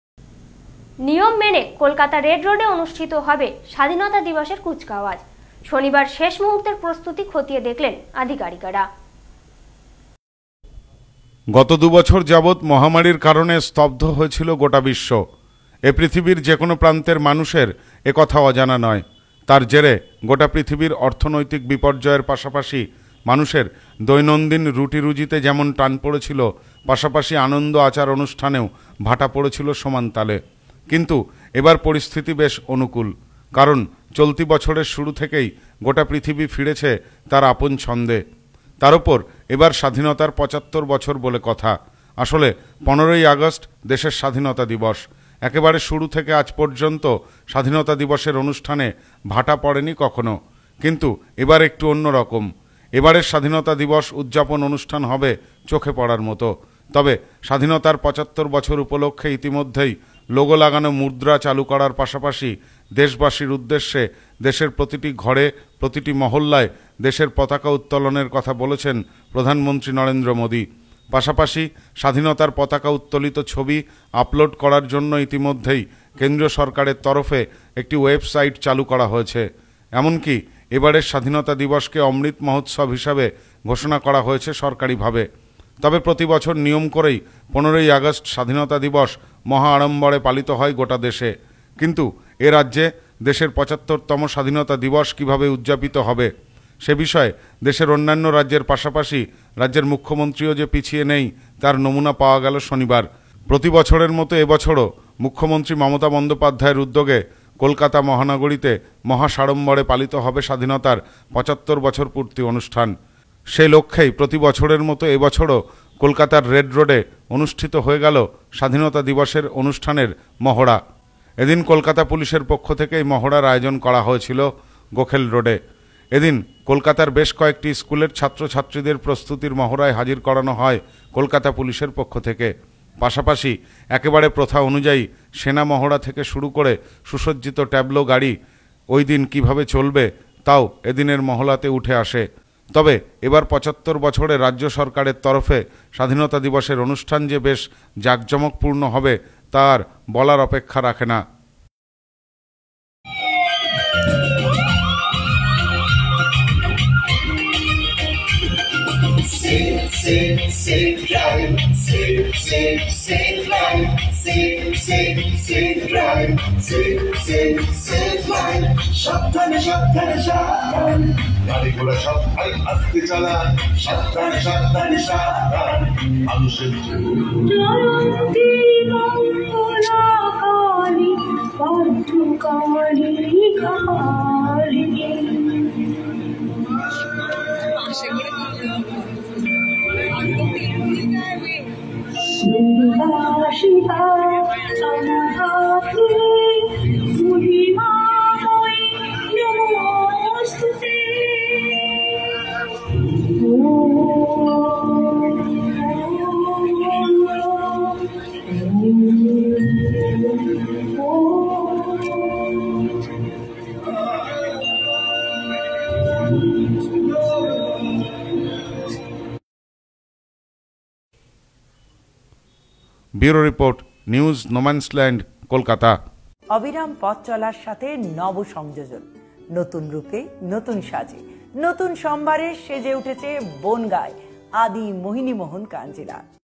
কলকাতা রেড রোডে স্বাধীনতা দিবসের কুচকাওয়াজের শেষ মুহূর্তের প্রস্তুতি।
নিয়ম মেনে কলকাতা রেড রোডে অনুষ্ঠিত হবে স্বাধীনতা দিবসের কুচকাওয়াজ। শনিবার শেষ মুহূর্তের প্রস্তুতি খতিয়ে দেখলেন আধিকারিকেরা |